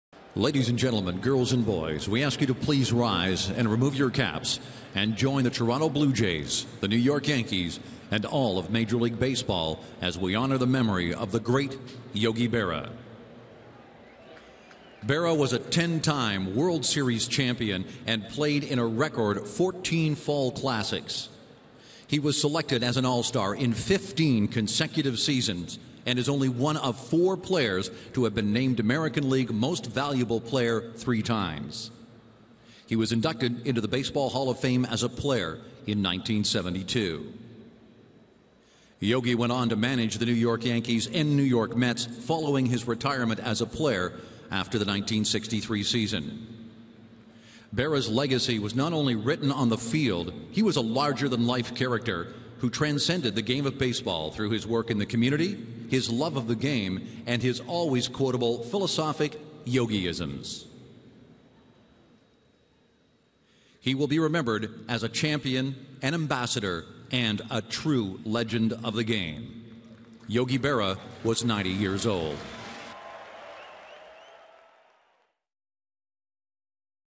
NYY@TOR: Blue Jays' booth honors Yogi Berra
9/23/15: The Blue Jays' booth honors Yogi Berra and touches on his career accomplishments